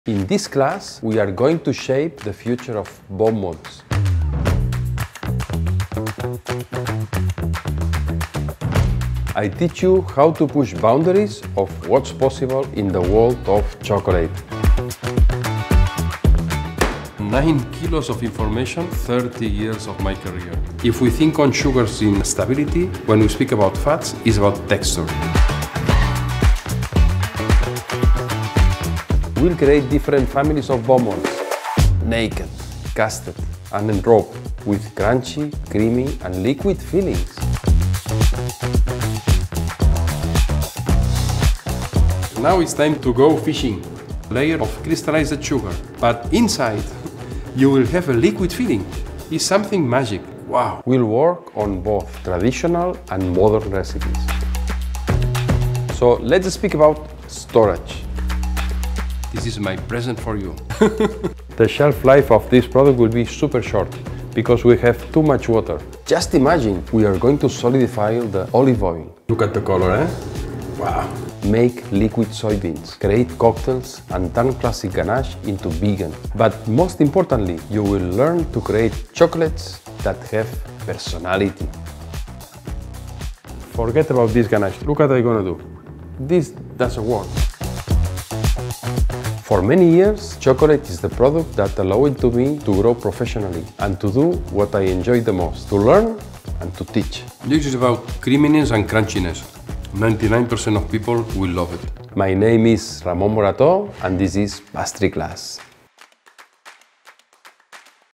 Ramon Morató Teaches Bonbons | Online PastryClass
Ramon Morató teaches you how to make casted, naked, and enrobed chocolates with liquid, ganache, creamy, and vegan fillings.

Learn to formulate your own ganache, molecular approach, various safety and storage practices that help you maintain the quality of your product and stay safe for your customer.

Ramon returns with his ✌🏻 second online PastryClass that will turn your chocolate to great success!